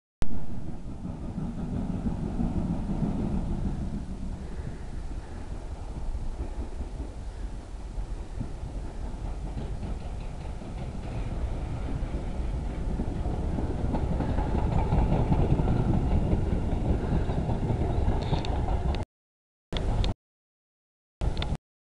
Pražský železniční den 24. 9. 2011 (1): Posun Ušaté
Parní lokomotiva 464 102 zvaná „Ušatá" přijela s historickým vlakem na smíchovské nádraží a posunuje se na druhý konec soupravy.